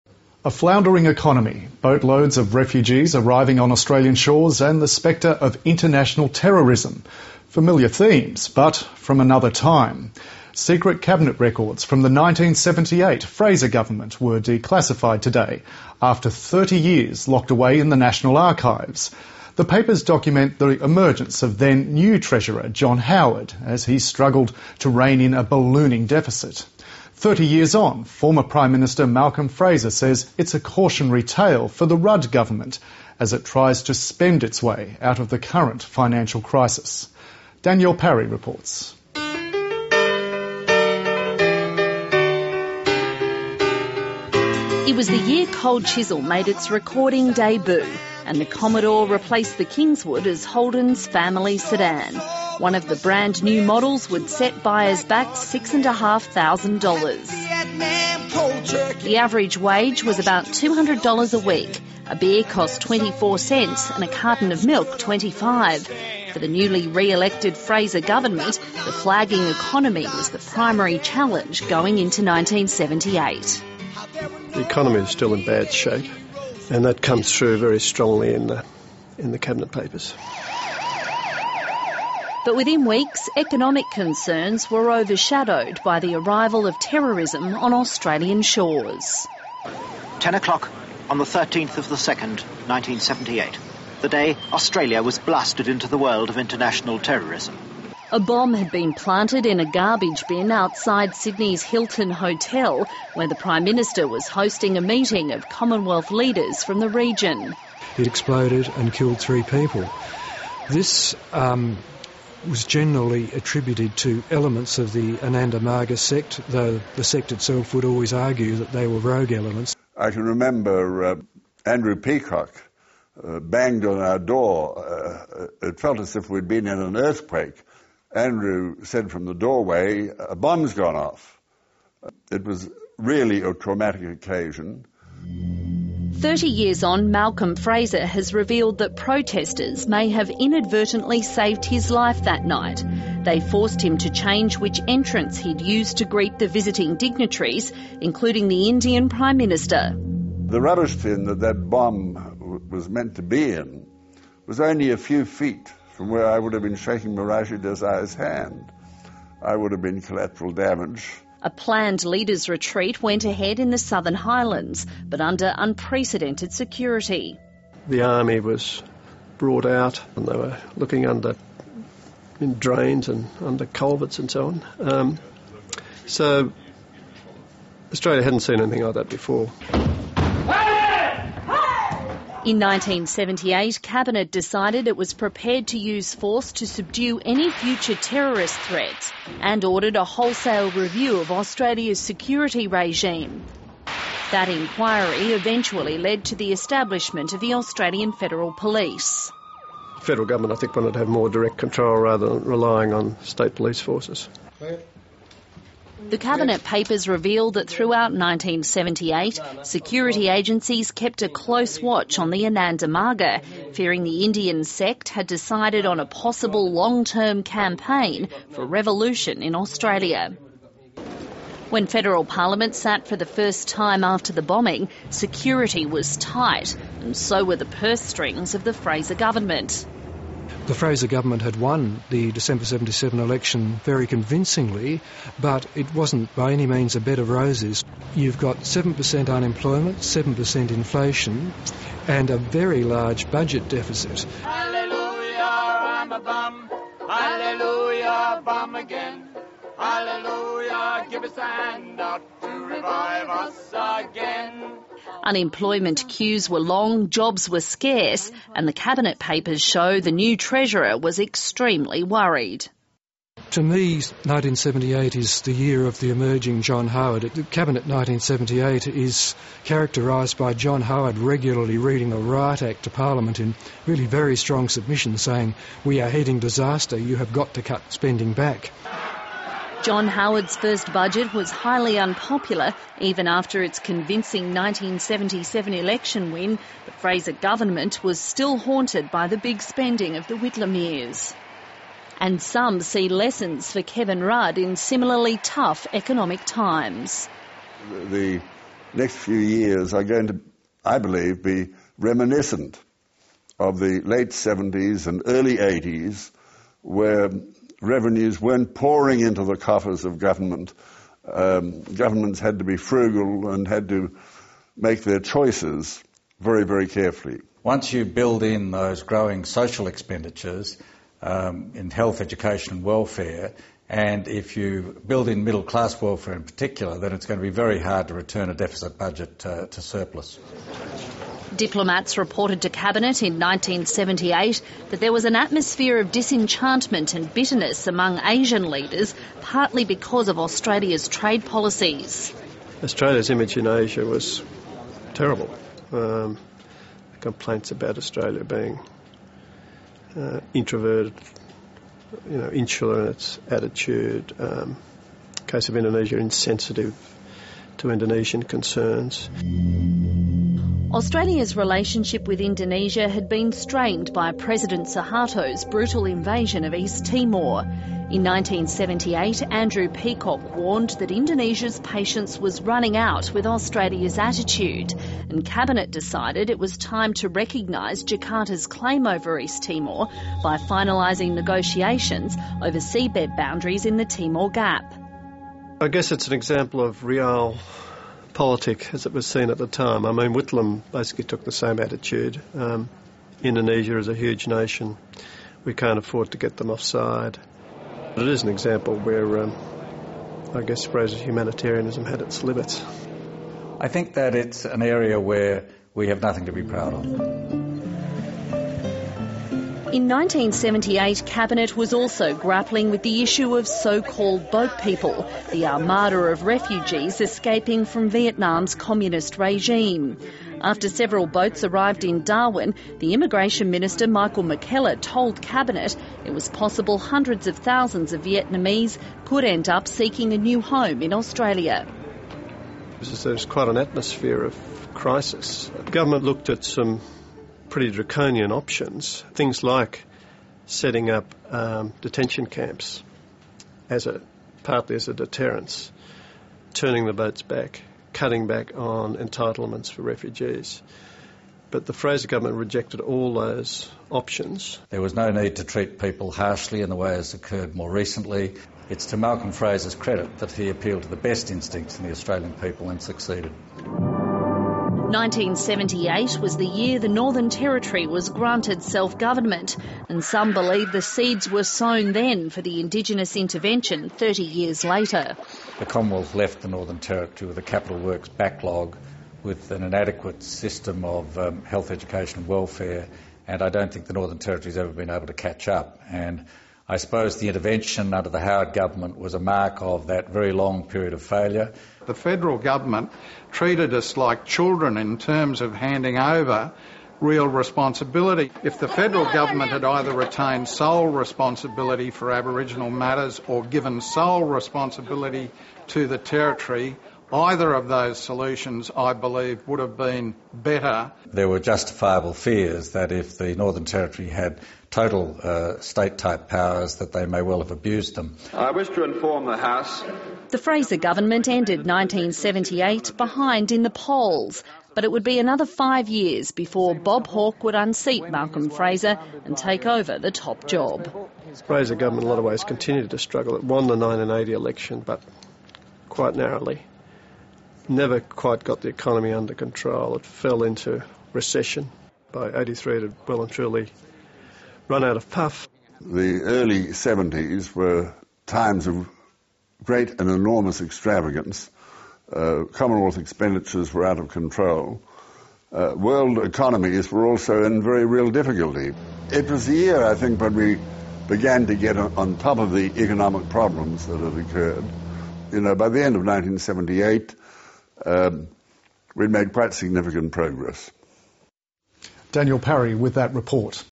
Secret Cabinet records from the 1978 Fraser government - Declassified
Secret Cabinet records from the 1978 Fraser government were de-classified after 30 years locked away in the national archives. The papers document the emergence of then treasurer John Howard, as he struggled to rein in a ballooning deficit. Malcolm Fraser says it is a cautionary tale for the Rudd Government as it tries to spend its way out of the current financial crisis.